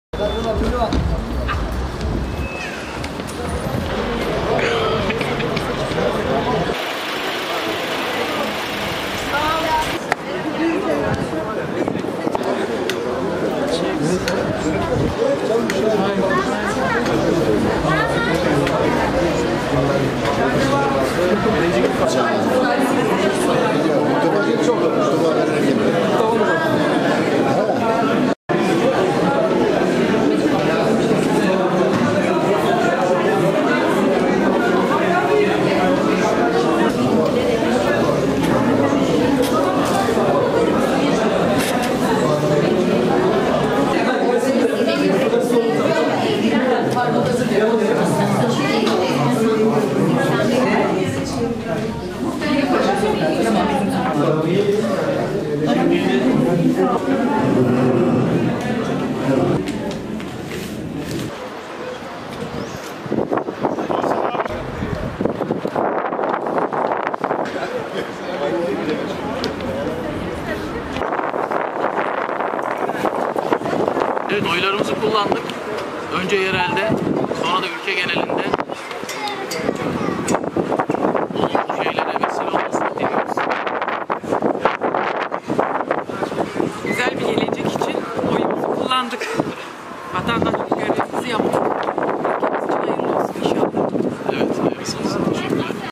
Silivri sandık başında!
Silivrili seçmenler, Mahalli İdareler Genel Seçimleri için sandık başına gitti. Seçimlerde, "Cumhur İttifakı" adı altında AK Parti ve MHP 51 ilde, CHP ve İYİ Parti de "Millet İttifakı" çatısı altında yine 51 ilde seçime giriyor. Büyük Birlik Partisi 30 büyükşehirde Cumhur İttifakı'nın adaylarını destekliyor. 30'u büyükşehir, 81 il, 922 ilçe ve 386 beldede 8 bin 270 adayın yarışacağı seçimlerde, vatandaşlar 5 yıl görev yapacak 1389 belediye başkanını belirleyecek